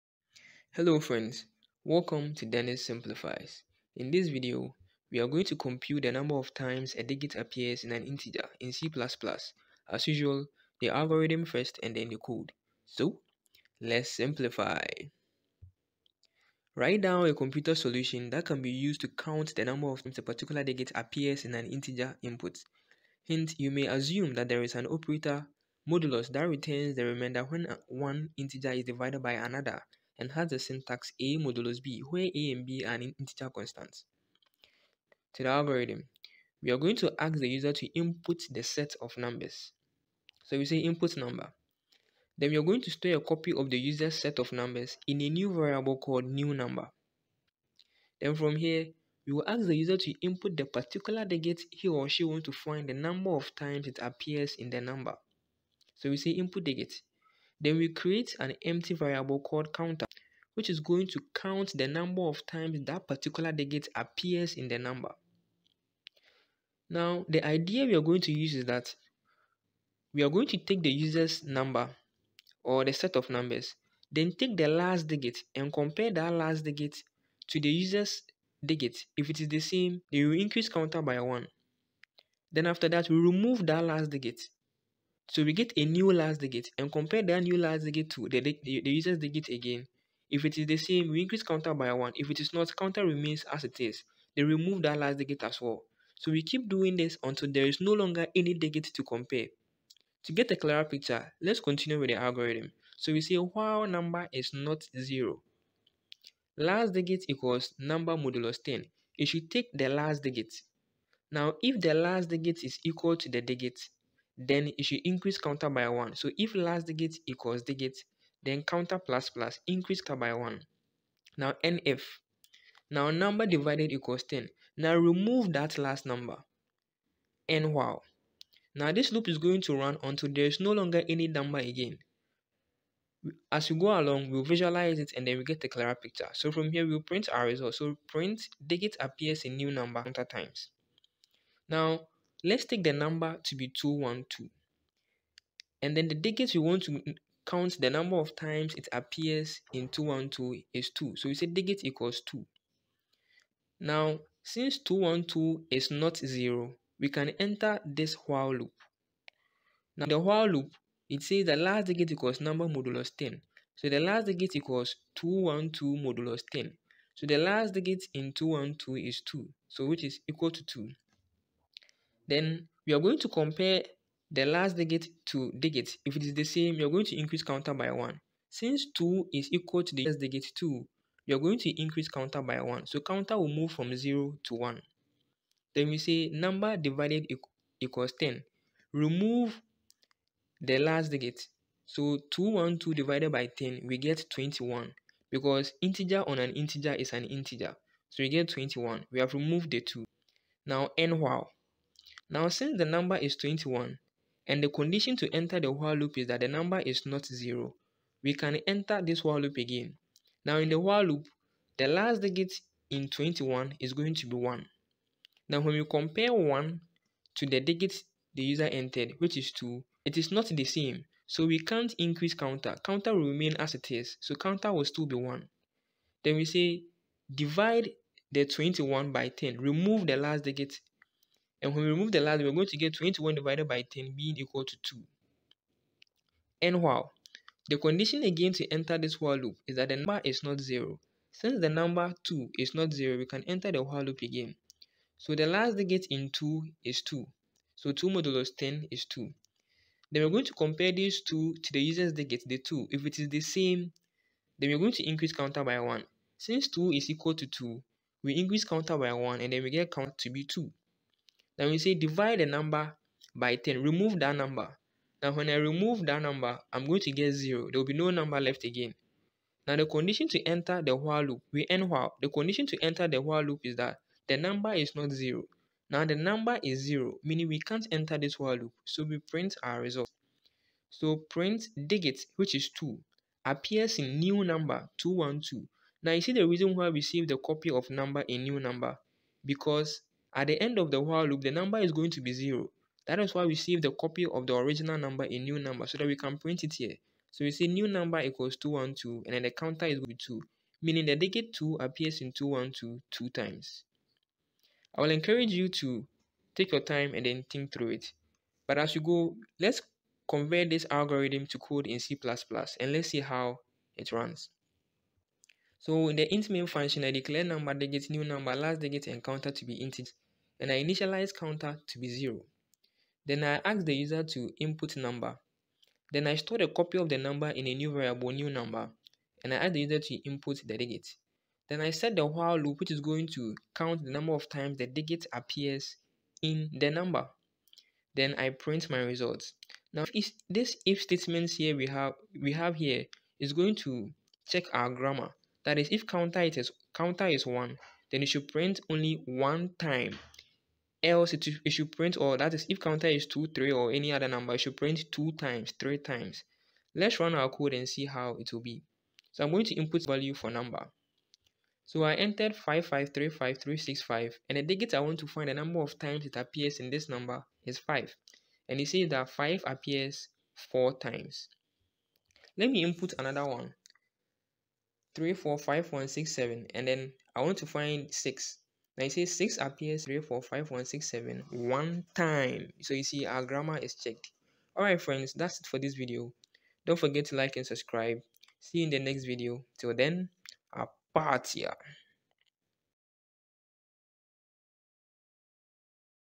The number of times a Digit appears in an Integer in C++ || With Algorithm
Let's Simplify
Computing the number of times a digit appears in an integer in C++